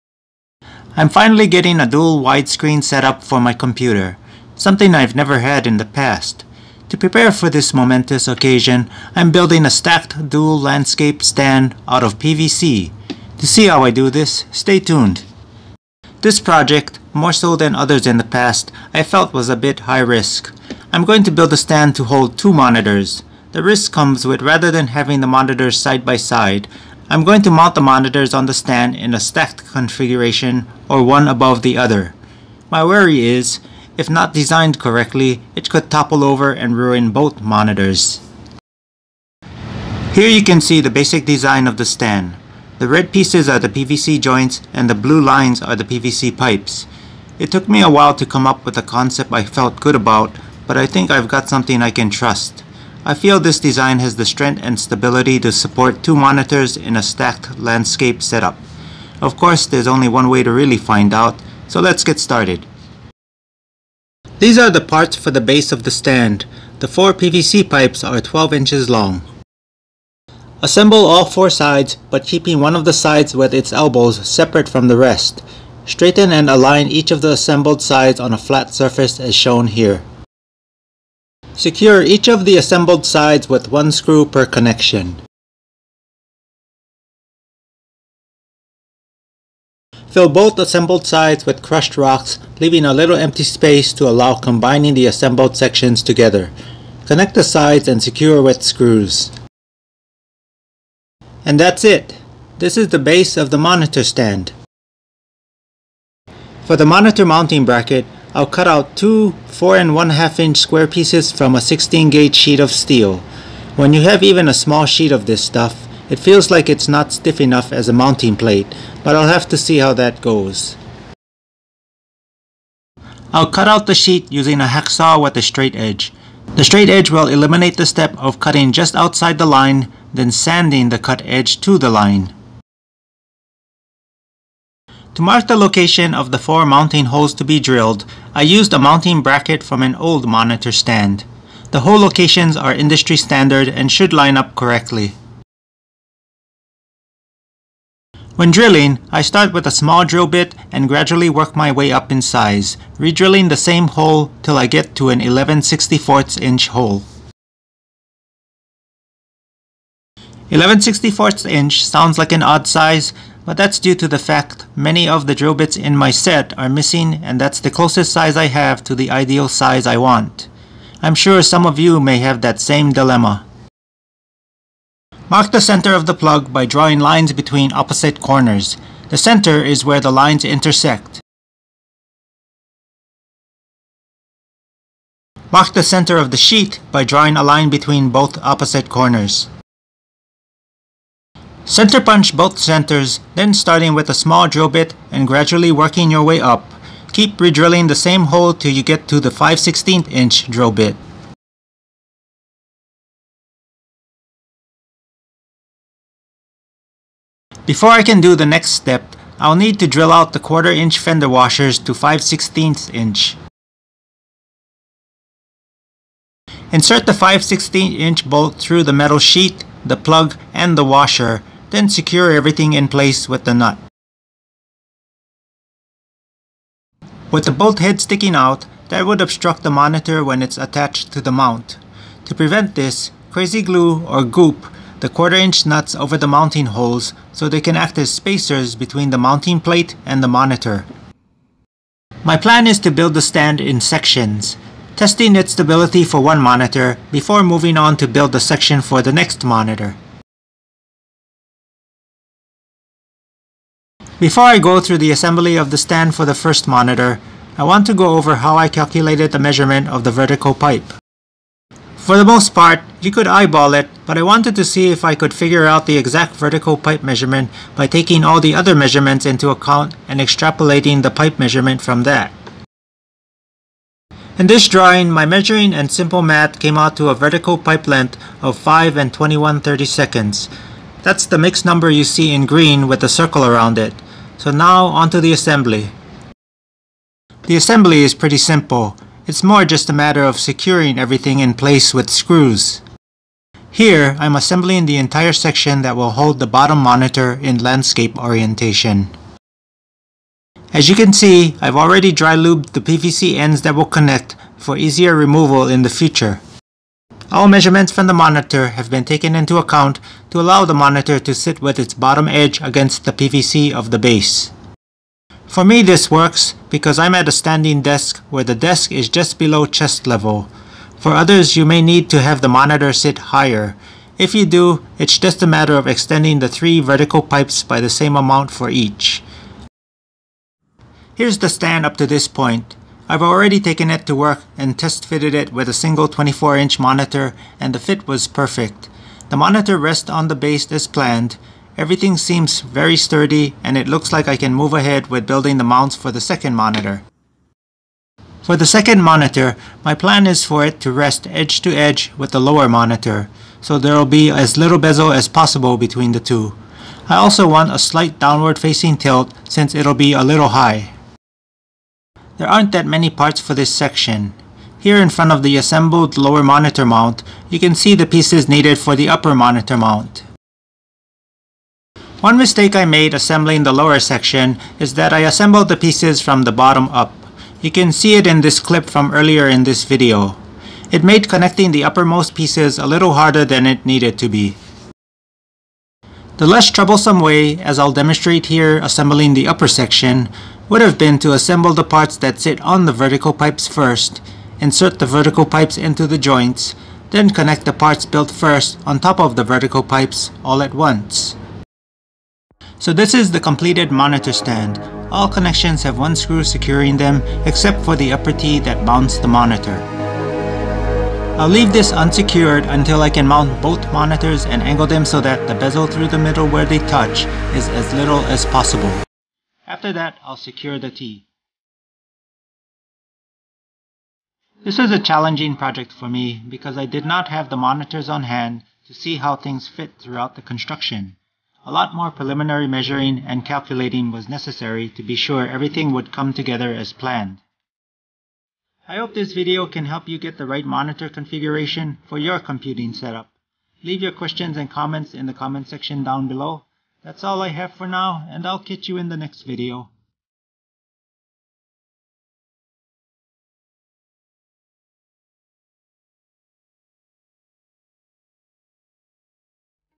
PVC Monitor Stand - Stacked Dual Landscape (DURABLE AND STURDY)
ORIGINAL POST:
I'm finally getting a dual wide screen setup for my computer.  Something I've never had in the past.  To prepare for this momentous occasion, I'm building a stacked dual landscape stand out of PVC.

PARTS LIST:
● 2x    4-1/2" x 4-1/2" square metal plates
● 2x    crushed rocks (Lowe's had a quart container of this)
● 10'    1-1/4" PVC pipe
● 7x    1-1/4" PVC elbow
● 6x    1-1/4" PVC tee
● 1x    1-1/4" PVC 4-way
● 2x    1-1/4" PVC hex head plug
● 1x    100 pk #10 x 3/4" pan head screws
● 2x    5/16" x 1" bolt
● 2x    5/16" nut
● 2x    1/4" fender washers
● 8x    1/4" nut
● 8x    M4 x 20 .70 pitch (these screws replaced the stock mounting screws - 1/4" longer)
● 1x    Goop glue

For reference, these are the measurements I took from my pair of Dell E2417H monitors:
- horizontal or vertical, between each mounting screw hole O.C. = 3-15/16
- mounting plate should be no larger than = 4-1/2" x 4-1/2"
- edge to edge monitor height (not screen) = 13-1/8"
- monitor bottom edge to top screw O.C. = 8-1/2"
- monitor top edge to top screw oc = 4-9/16"

Intro music track provided by the Frequency channel on YouTube.
Download link to all music is in each song's description.

Outro music track provided by the Frequency channel on YouTube.